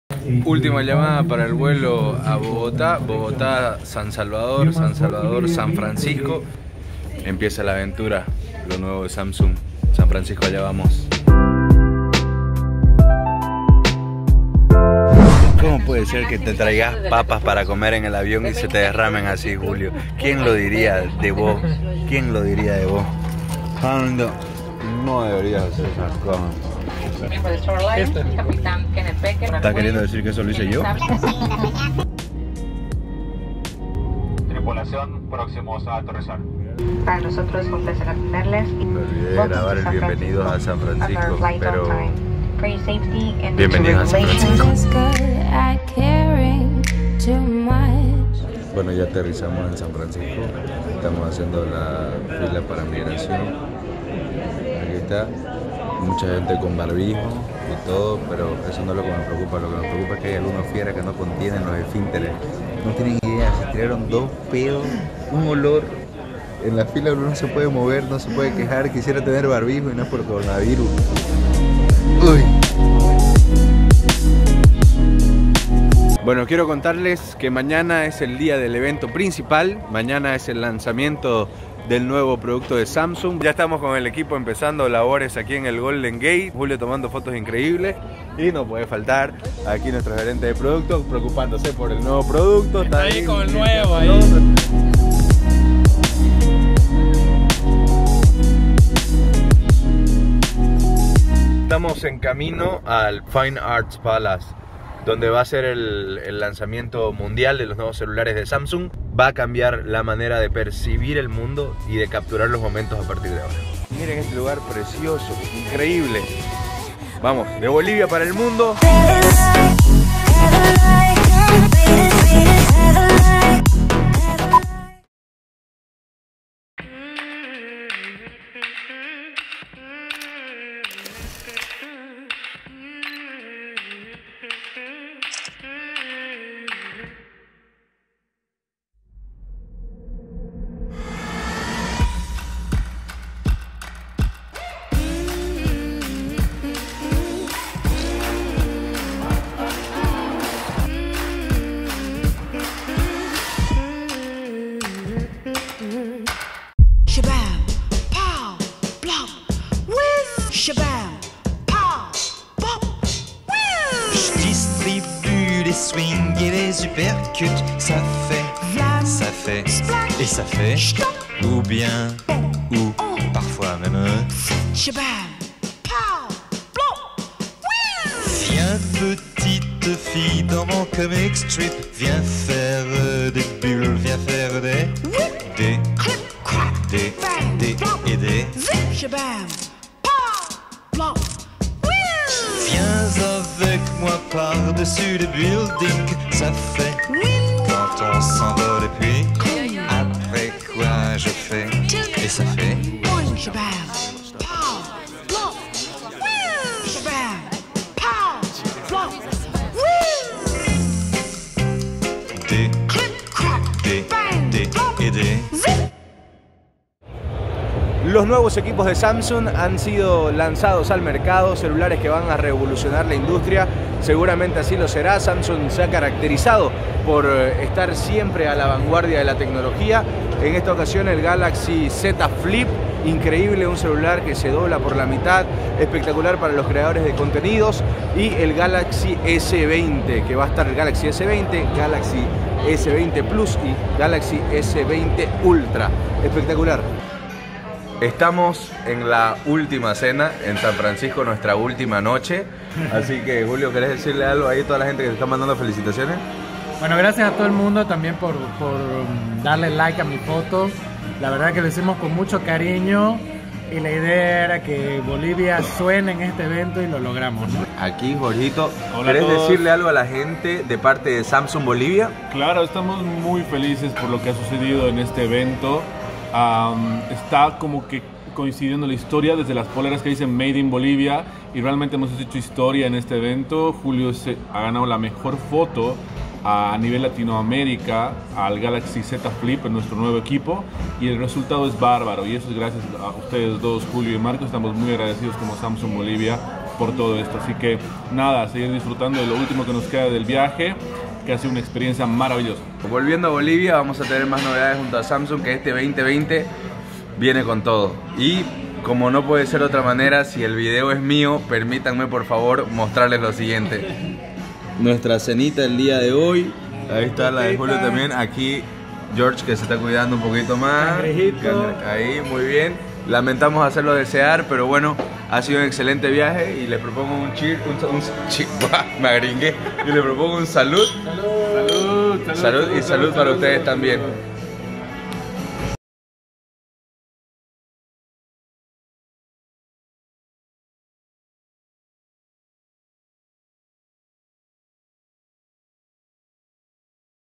Samsung Unpacked en San Francisco
Mi primer Vblog, les comparto nuestra aventura en San Francisco junto a Samsung!, pronto el Vblog de la visita a Alcatraz!😱